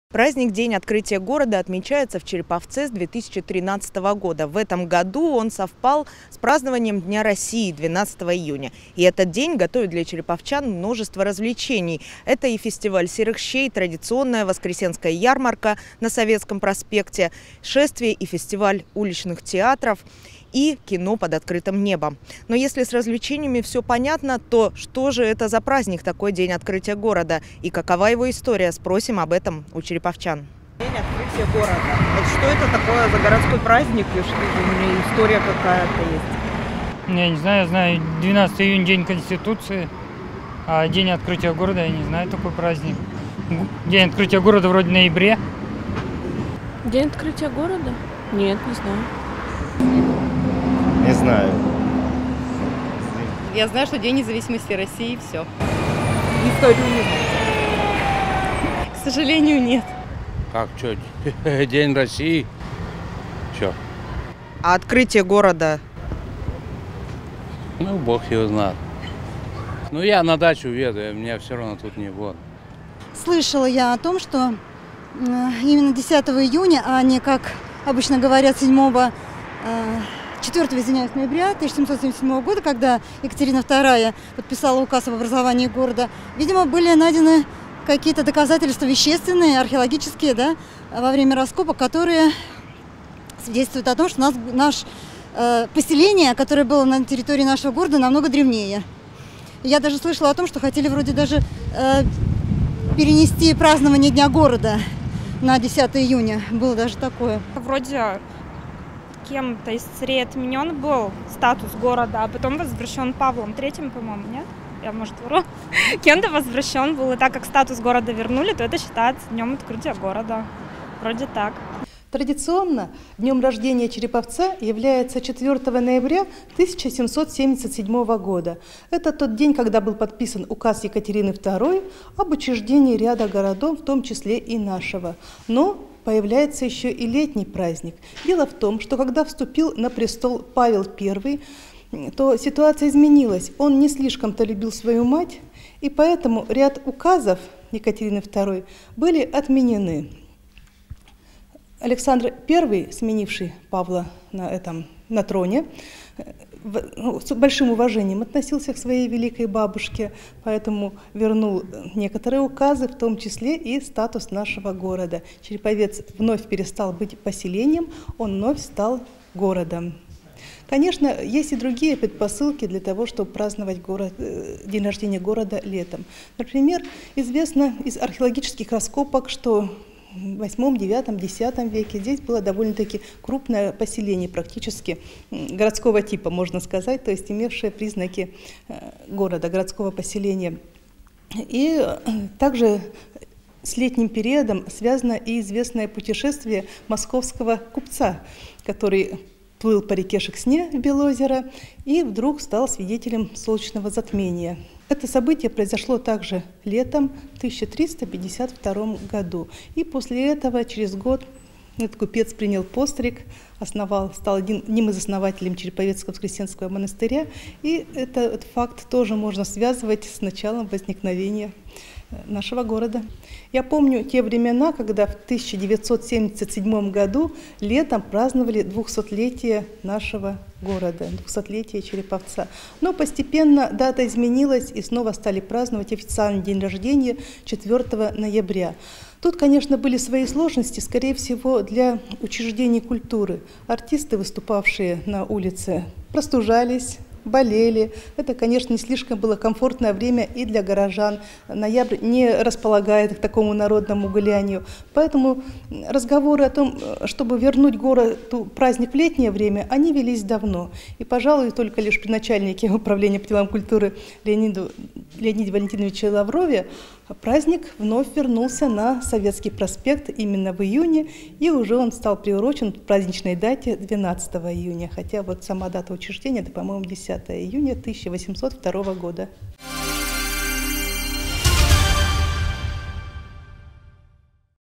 «День открытия города? Нет, не слышал»: история молодого праздника Череповца
Праздник День открытия города отмечается в Череповце с 2013 года. В этом году праздник пройдёт 12 июня, вместе с Днём России. Город готовит череповчанам множество развлечений – традиционная Воскресенская ярмарка на Советском проспекте, шествие и фестиваль уличных театров, кино под открытым небом, дегустация серых вологодских щей и многое другое. Но что это за праздник такой – День открытия города и какова его история?